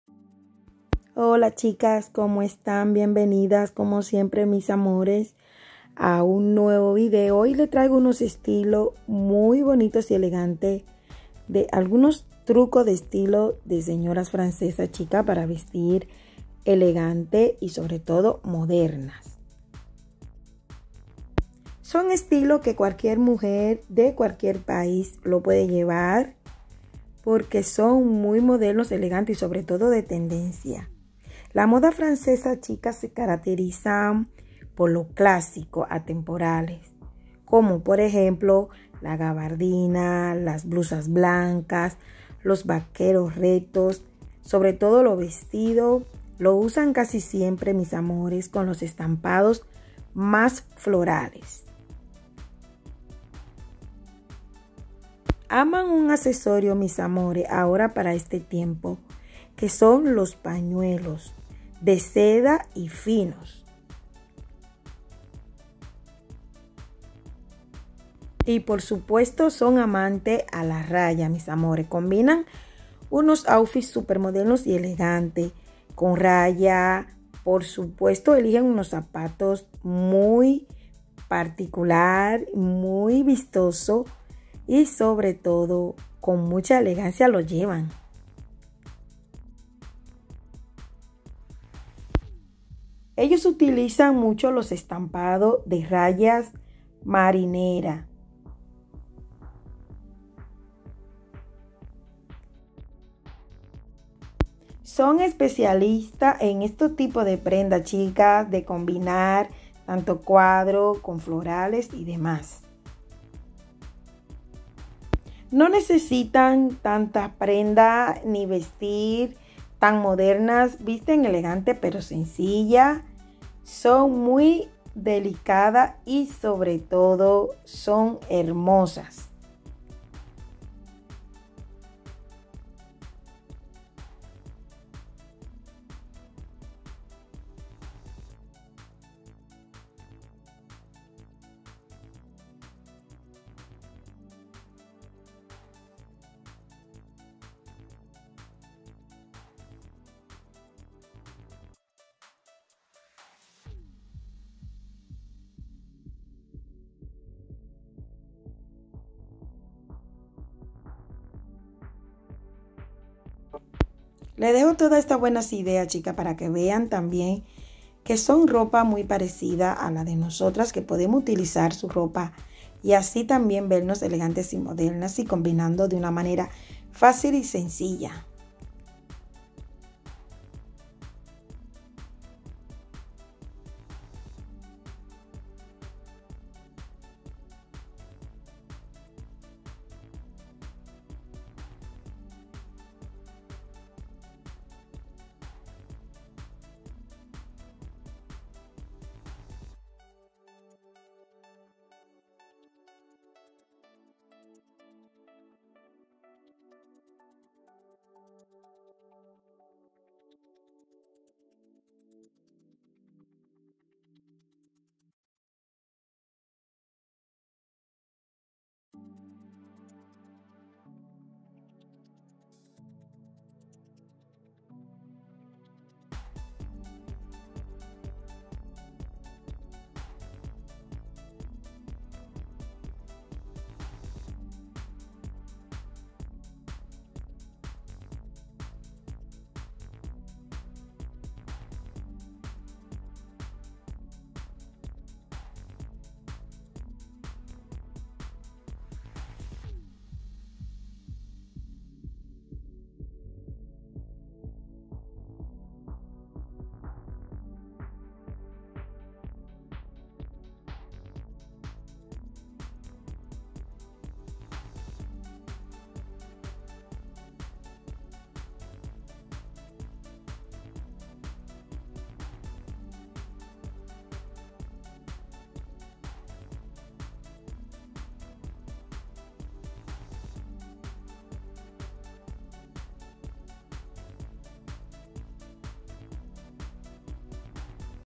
25 TRUCOS de ESTILOS DE MUJERES FRANCESAS para vestir ELEGANTE Y MODERNAS a los 40 50 y 60
CÓMO VISTEN LAS MUJERES FRANCESAS EN DIFERENTES ESTILOS CÓMODOS Y MODERNOS